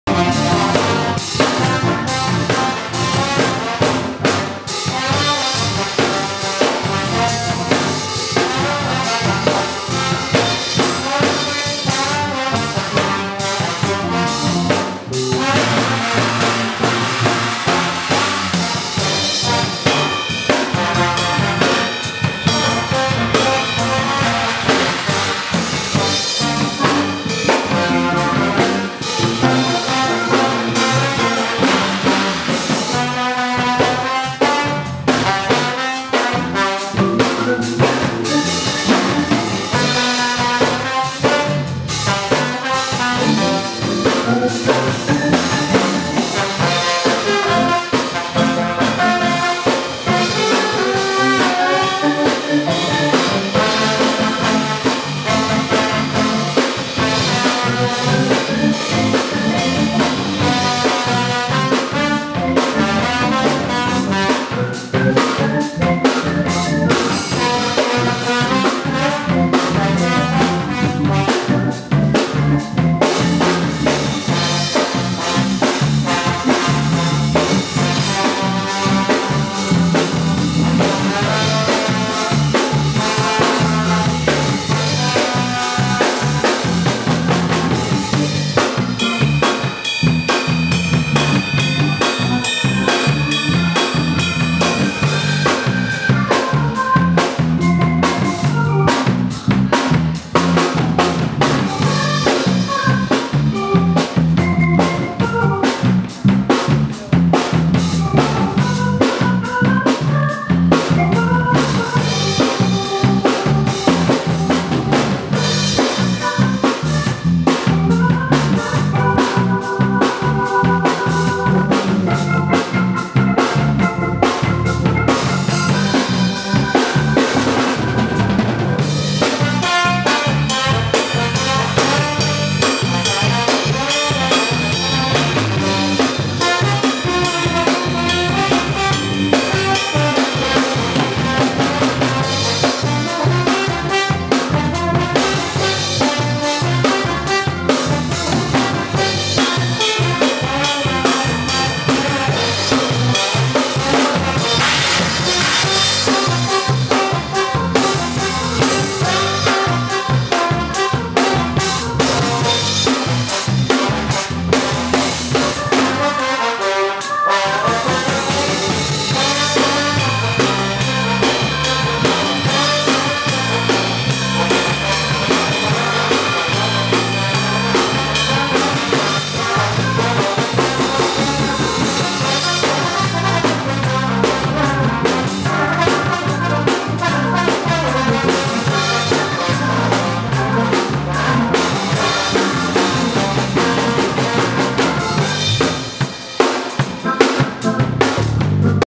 Suavo J ft Mr.88 & the Bones (with special guest Arean Alston)
Live performance at Center for Southern Folklore. Memphis, TN. 3 January 2013.